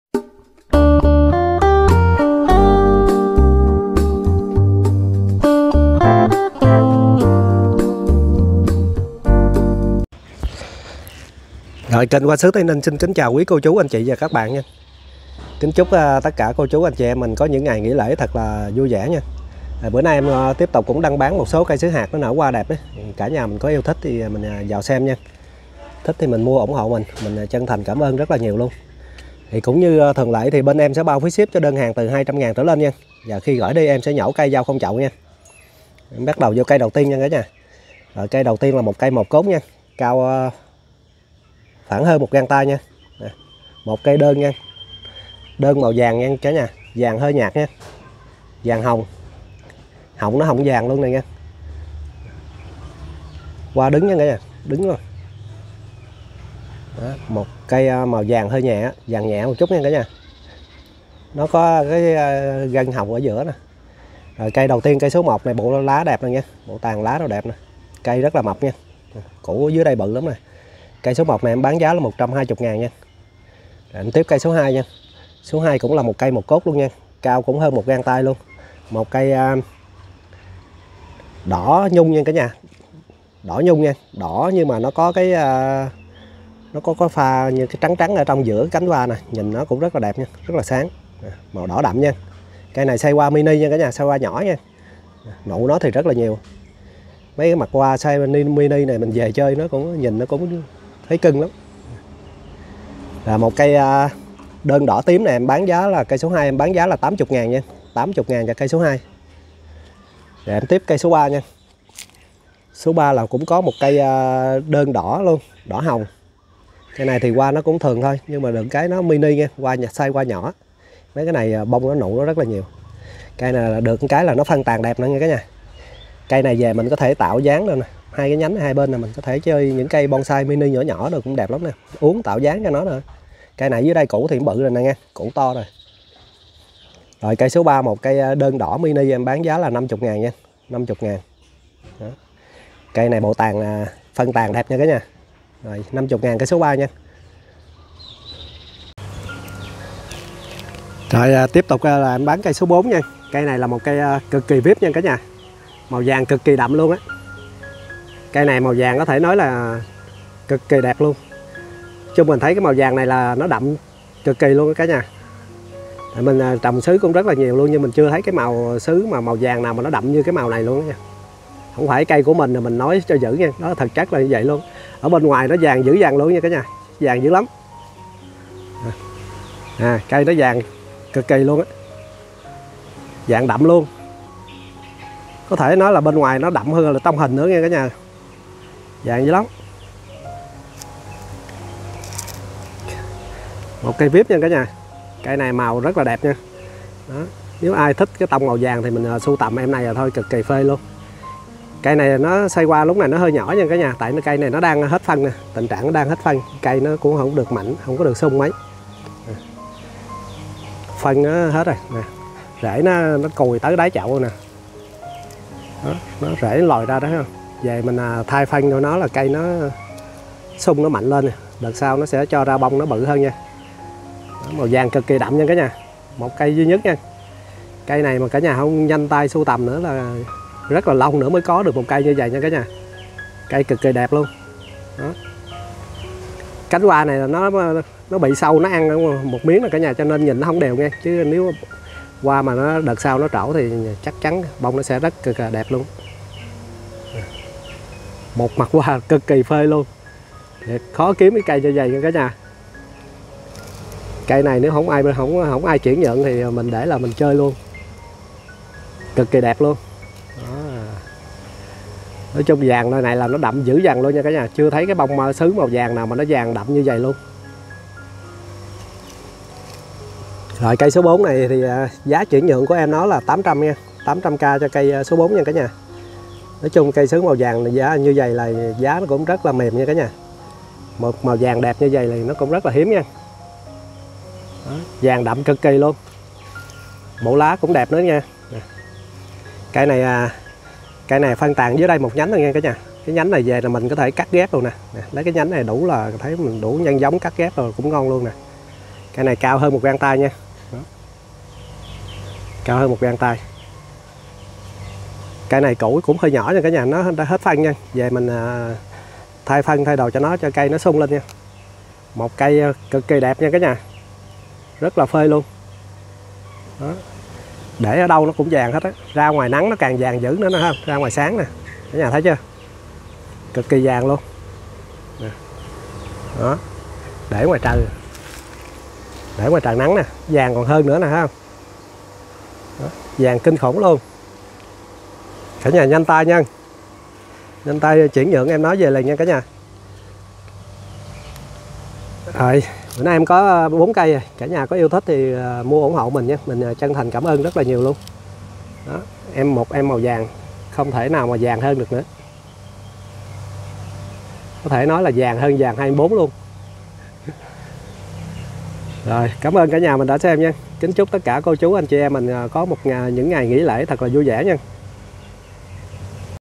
Sứ hạt lai tạo | Cây màu vàng vip | Hoa sứ tây ninh | Đã bán
Bán sứ hạt lai tạo hoa xinh có cây màu vàng vip